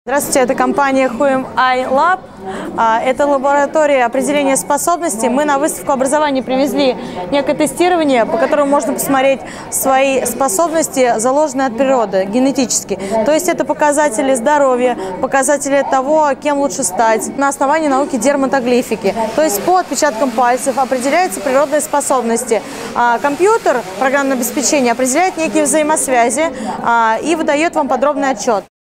Тест для определение генетических способностей компании WHOAMI, Образовательная среда - 2013
Компания WHOAMI занимается профориентацией  согласно генетически заложенных способностям и потенциалу

Выставка: Образовательная среда - 2013